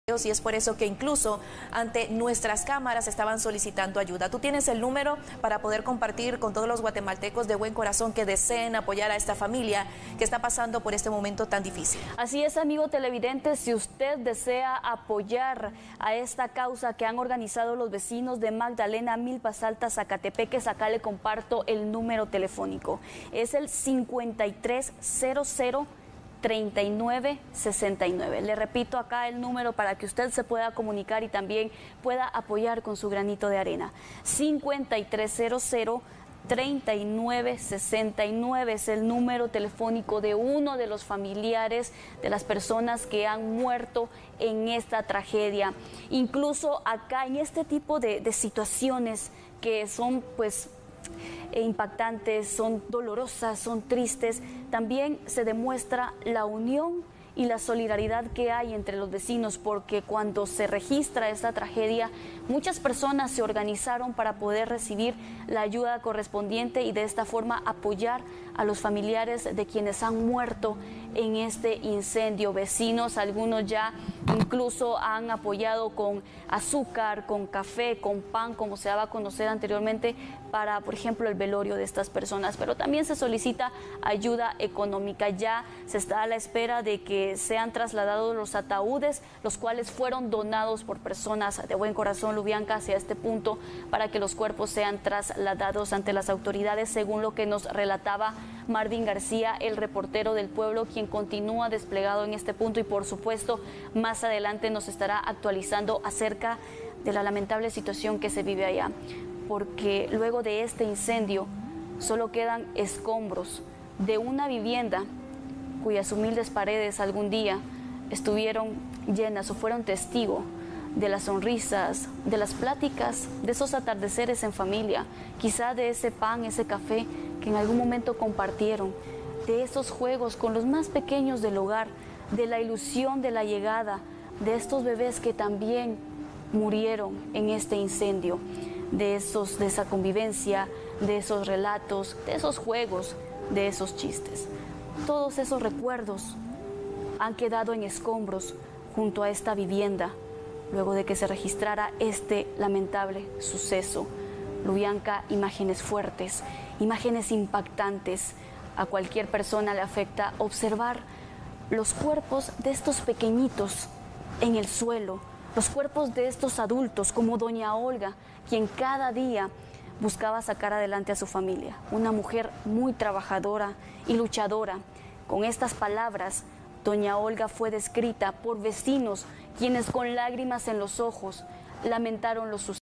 ¿Cómo ayudar ante la tragedia de Magdalena Milpas Altas?
Si usted desea brindar ayuda a los familiares de las personas que perdieron la vida en la tragedia de Magdalena Milpas Altas puede llamar al: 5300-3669